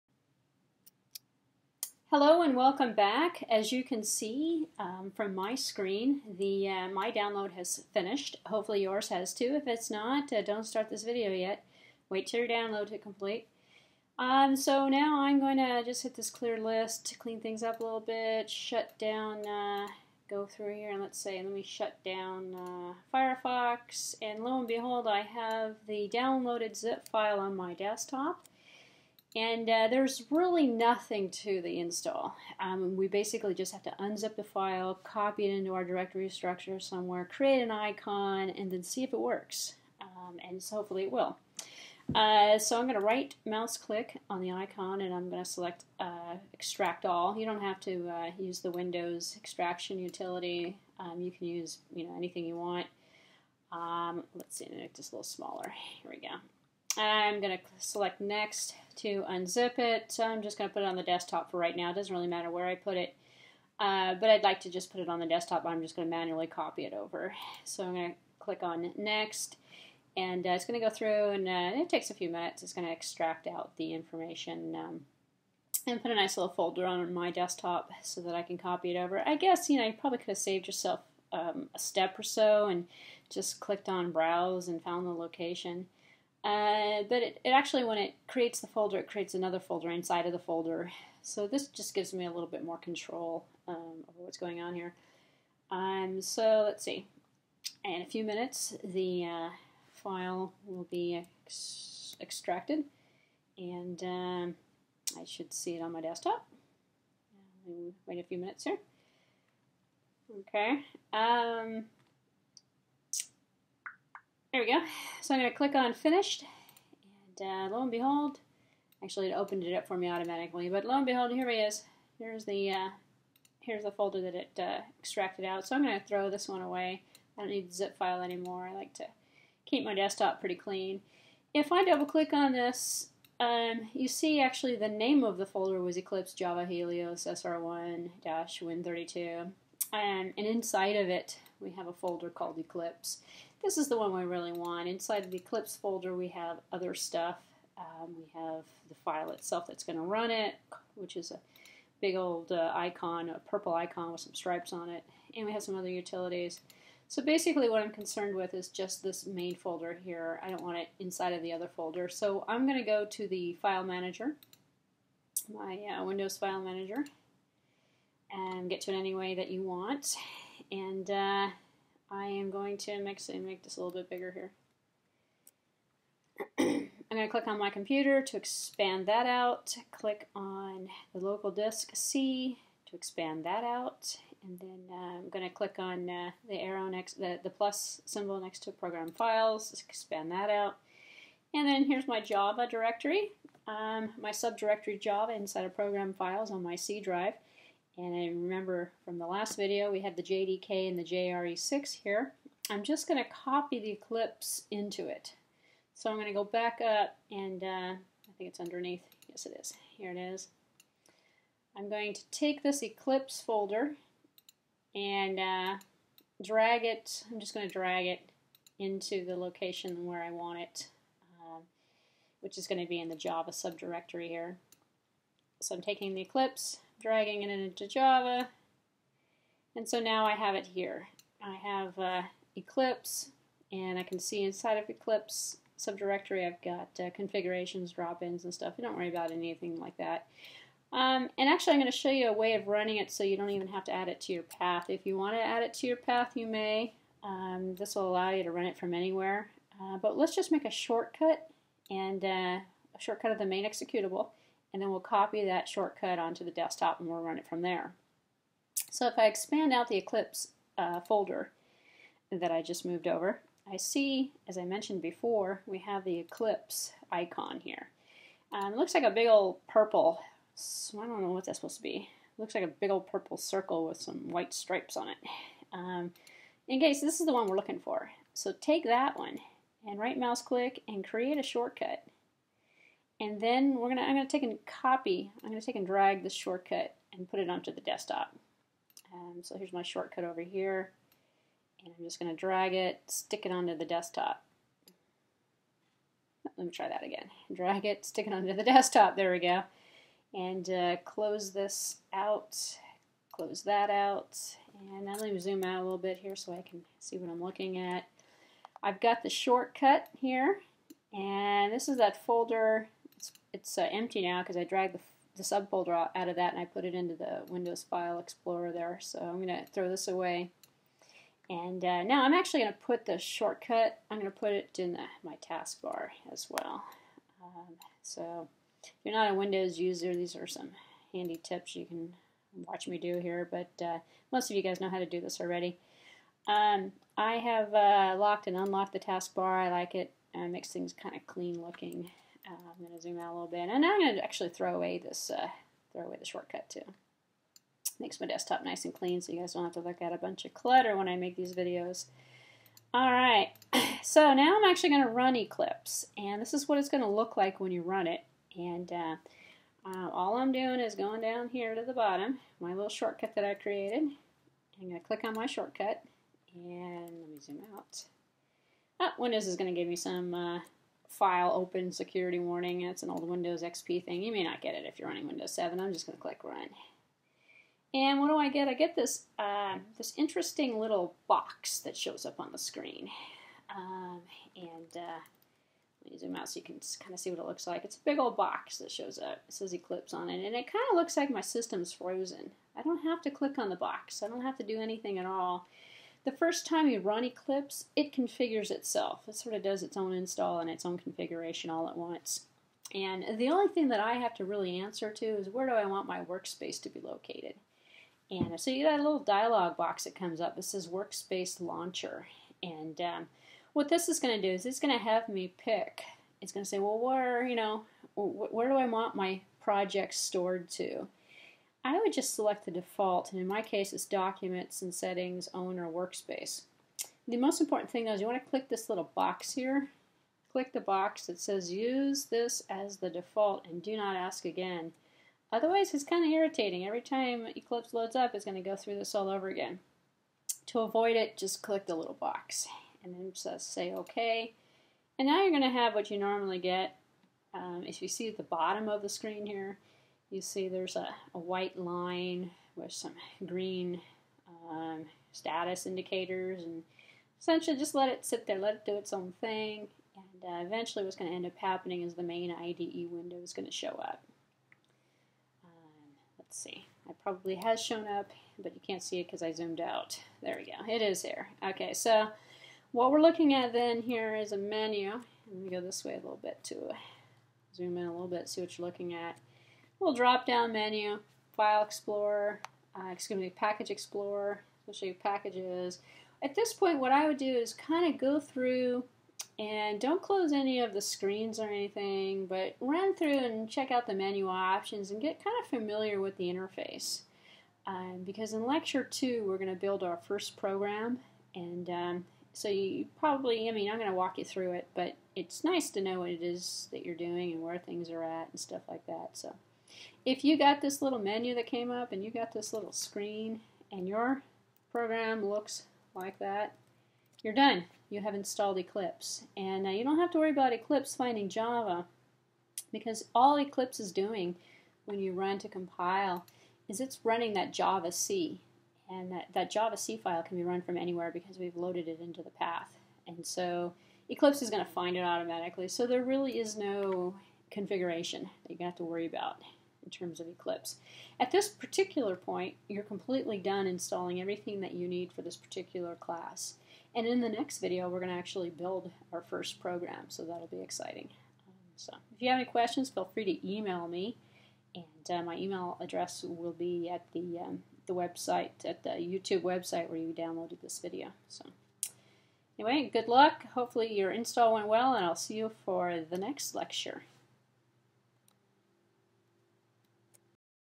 OO Design in Java - Tutorial 1 Part 5
This video shows you how to install Eclipse.